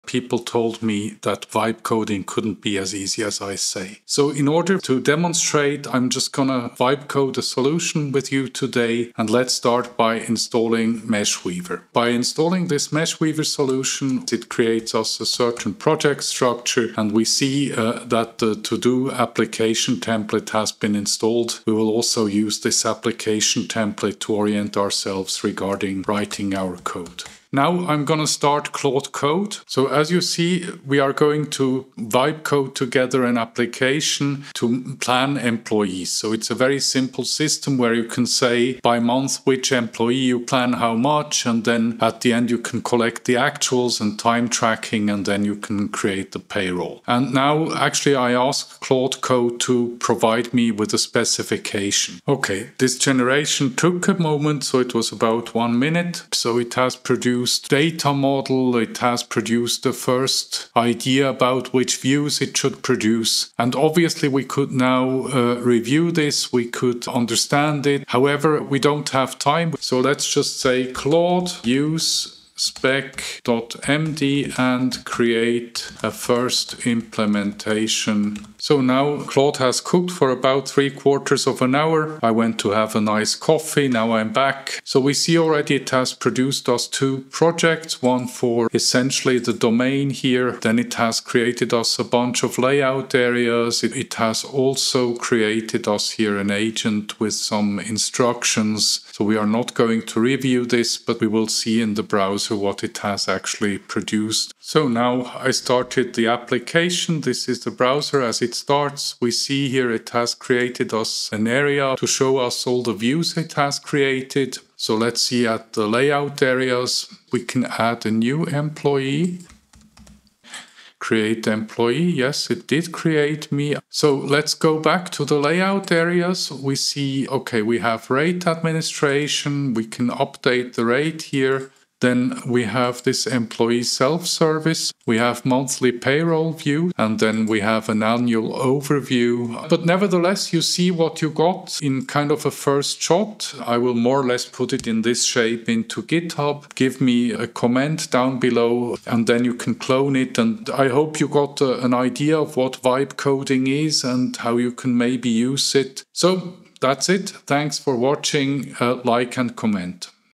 Business Application Vibe Coding
See how you can vibe code a business application in November 2025.

MeshWeaver
An Open Source Technology Connecting Your Data in a Data Mesh to AI Tools